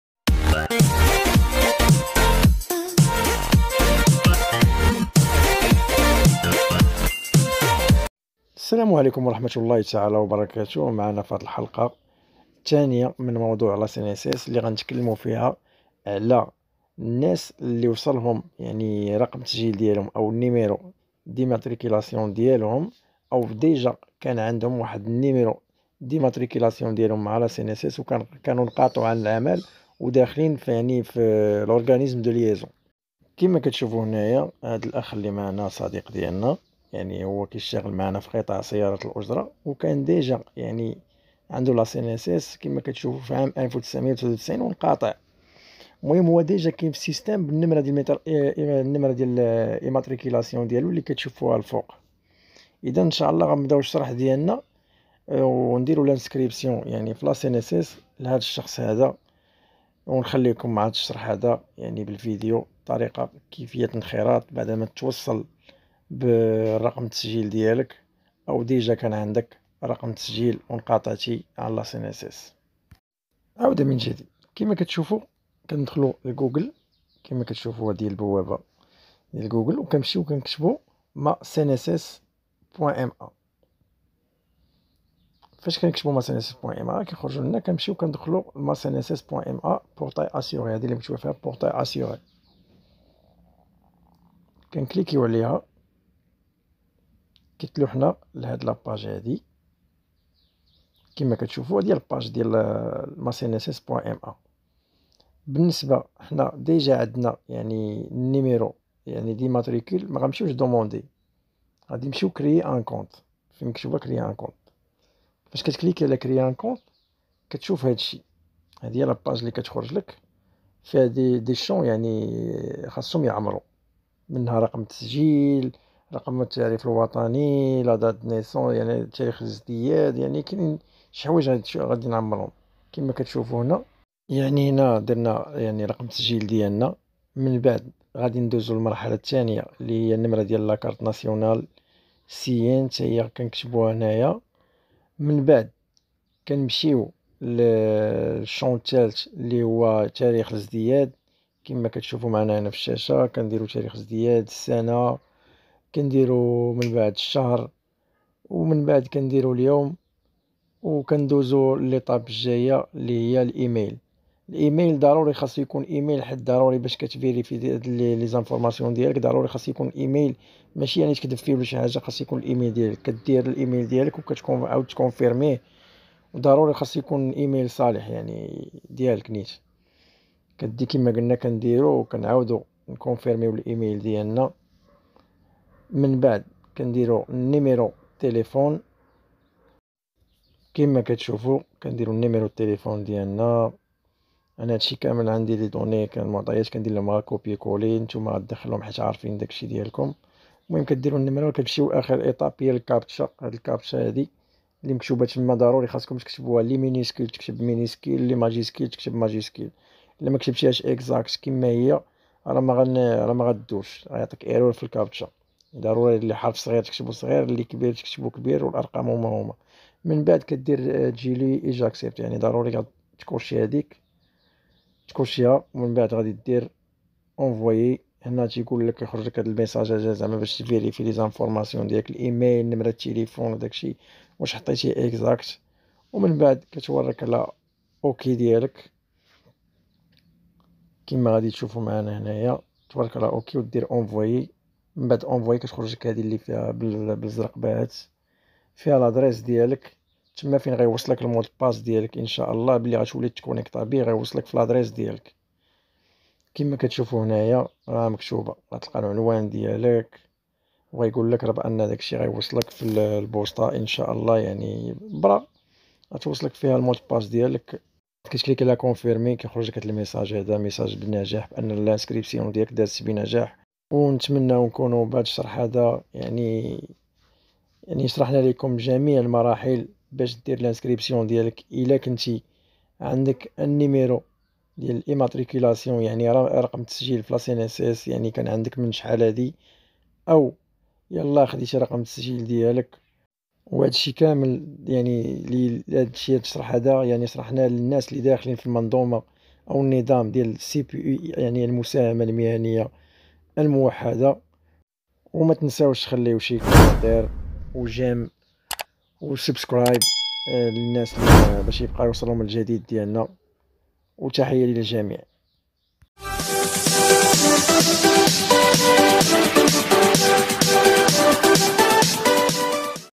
كيفية الانخراط في الضمان الاجتماعي بالنسبة للاشخاص اللذين يتوفرون على رقم التسجيل المنخرطين في cpu
كيفية الانخراط في cnss بالنسبة للاشخاص المنخرطين في نظام cpu المتوفرين على رقم التسجيل numero immatriculation

بالنسبة للاشخاص الغير المتوفرين على رقم التسجيل
يشوفو هاد الفيديو